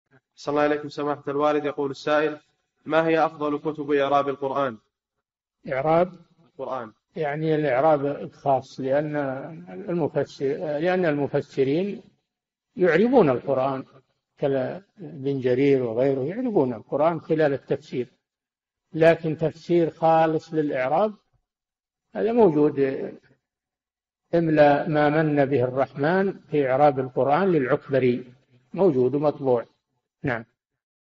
[174] أفضل كتب إعراب القرآن الكريم - الشيخ صالح الفوزان
"[4717-174] ما هي أفضل كتب إعراب القرآن؟
الجَوَابُ: يعني الإعراب الخاص لأن المفسرين يعربون القرآن، كابن جرير وغيره يعربون القرآن خلال التفسير، لكن تفسير خالص للإعراب، هذا موجود [إملاء ما منّ به الرحمن في إعراب القرآن] للعكبري، موجود ومطبوع.

للاشتراك في قناة الشيخ صالح الفوزان
فتاوى المجالس في تفسير المفصل

للاستفسار والإقتراحات والدعم
رئيس الفريق
لمشروع كبار العلماء